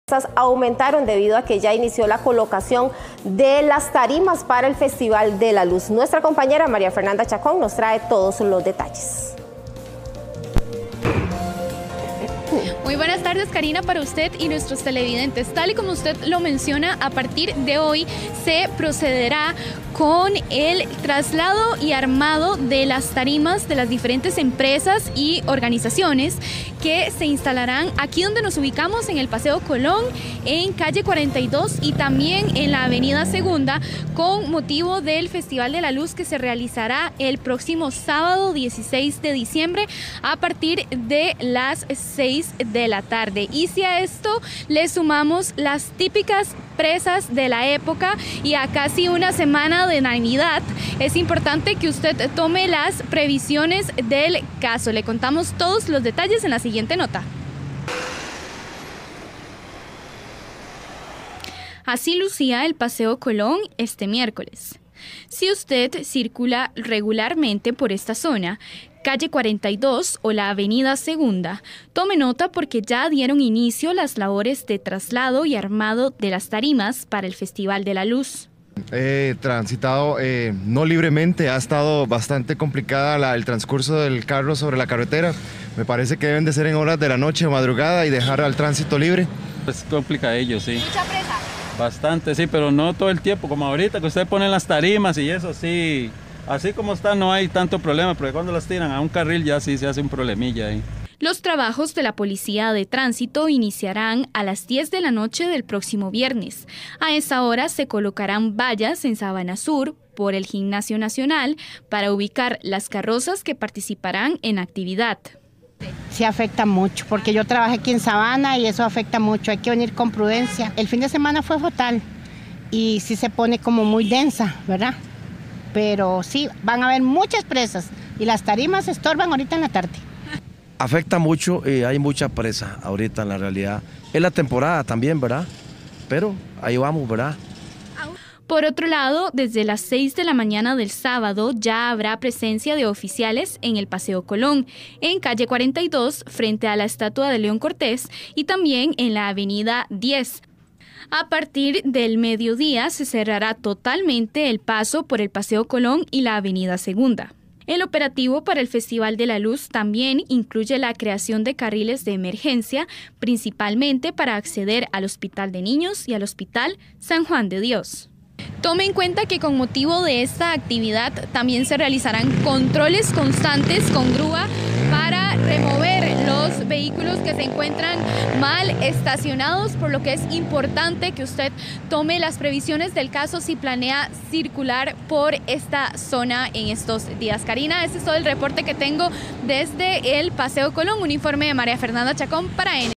Ya inició la colocación de las tarimas para el Festival de la Luz
Advierten que armado de estructuras generará presas en las vías aledañas a la zona del evento.